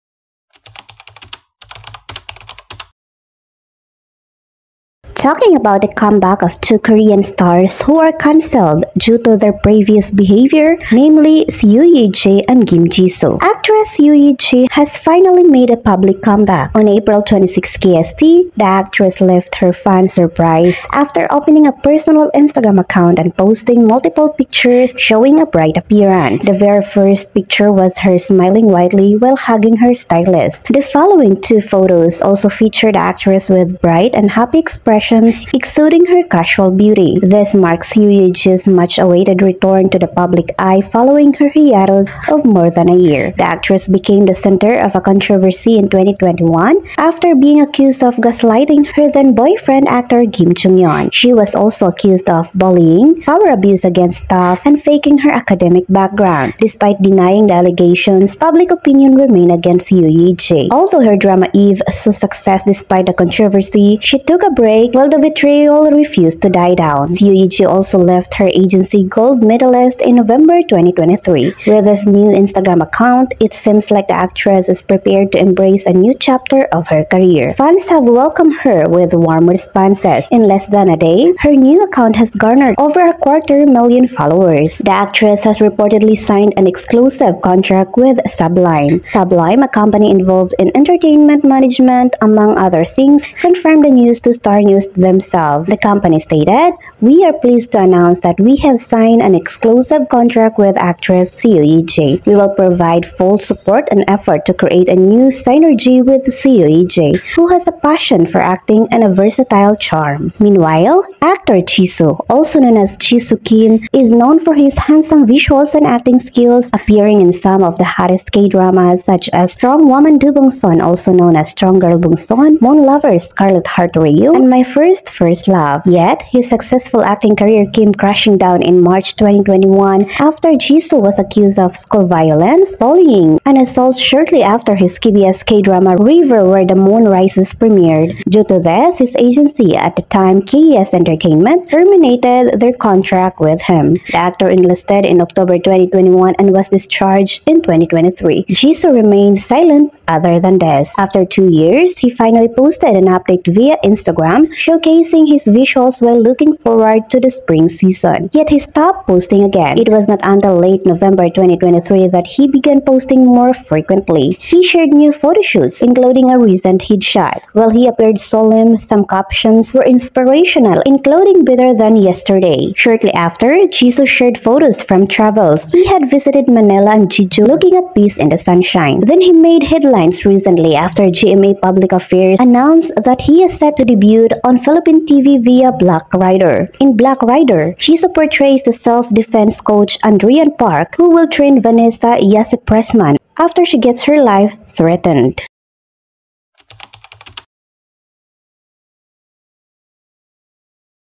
After CONTROVERSIES ~ Kim Jisoo and Seo Yea Ji MADE A STUNNING COMEBACK in the entertainment.
After CONTROVERSIES ~ Kim Jisoo and Seo Yea Ji MADE A STUNNING COMEBACK in the entertainment. Although Jisoo set in Philippine TV.